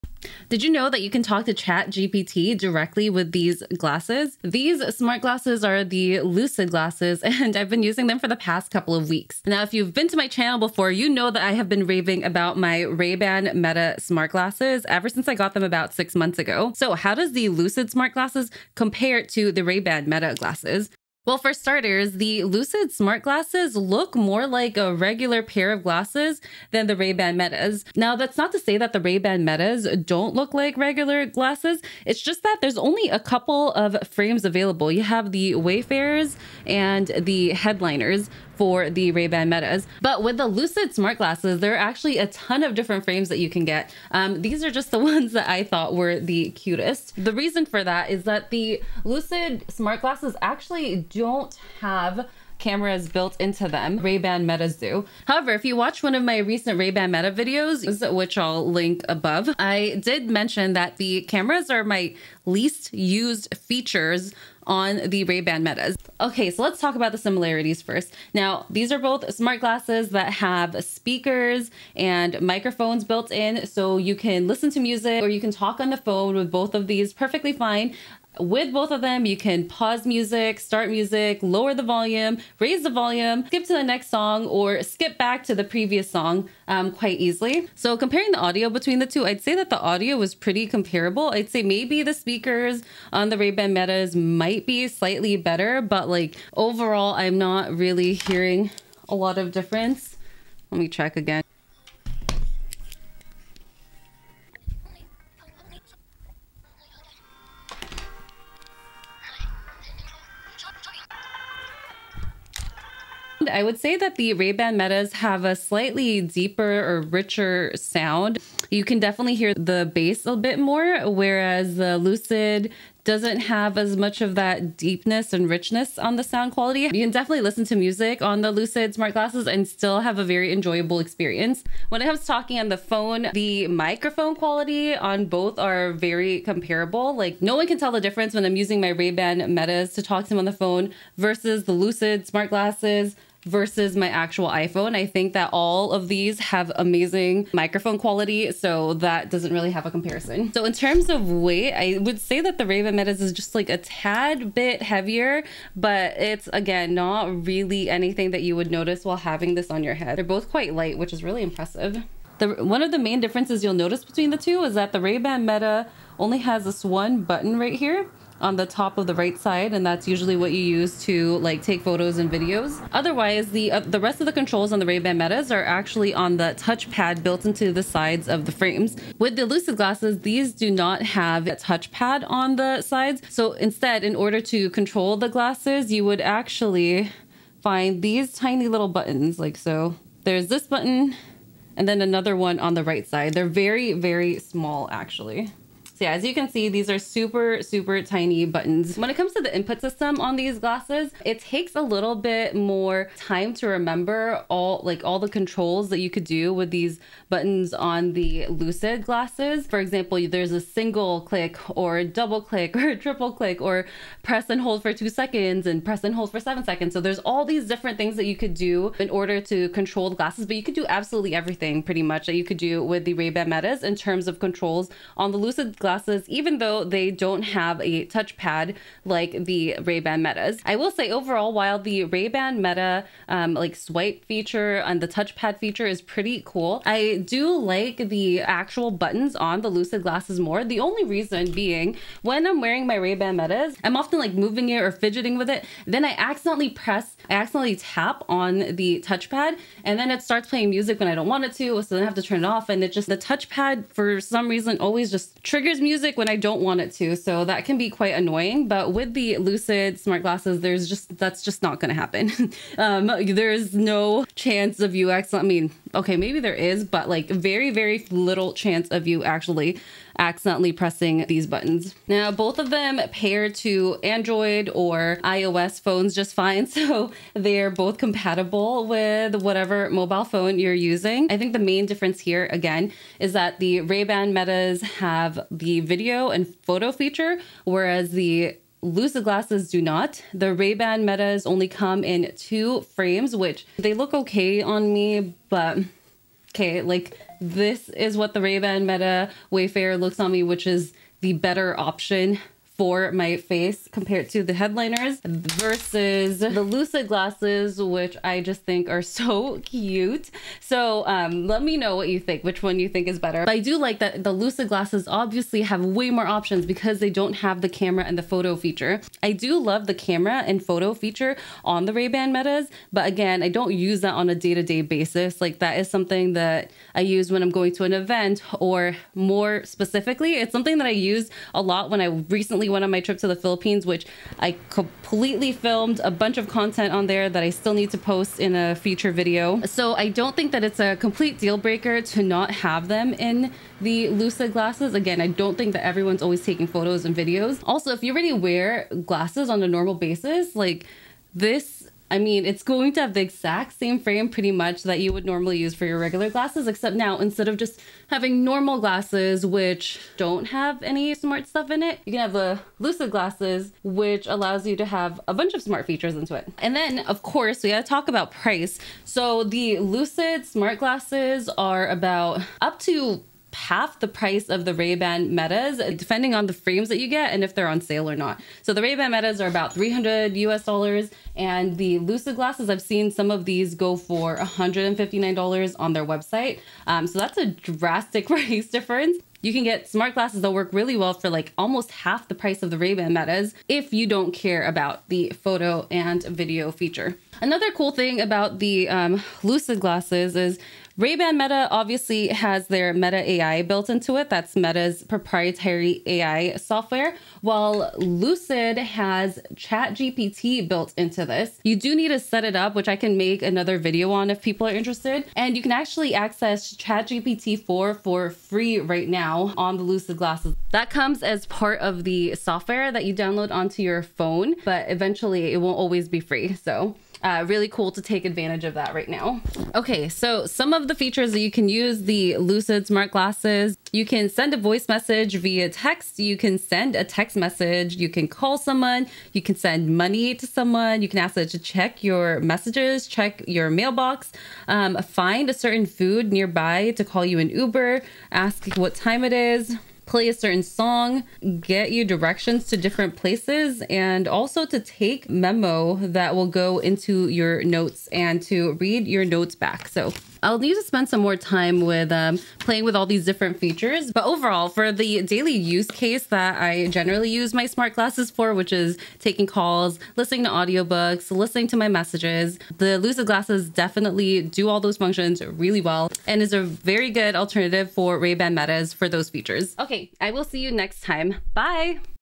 Half the Price, But Is It Worth It? (Smart Glasses)
Ray-Ban Meta VS. Lucyd Smart Glasses

After weeks of using the Lucyd Smart Glasses, I’m excited to share my personal experience, including comparisons and it’s standout features.  Rayban Meta Smart Glasses or Lucyd Smart Glasses? I'll help you decide which one best suits your needs!

📨 hello [at] vrwithjasmine [dot] com

⏰Time Stamps⏰

00:00 Comparison
00:17 Frames
01:07 Similarities
01:28 Audio
2:32 Weight
3:06 Controls
4:32 Glasses vs Smartphones
5:02 Camera
6:07 Price
6:39 Smart glasses vs Regular Glasses
7:10 additional features
7:40 Overall experience

*Thank you for using my links and supporting this channel 💙

✨ Amazon Storefront - Recommended VR Products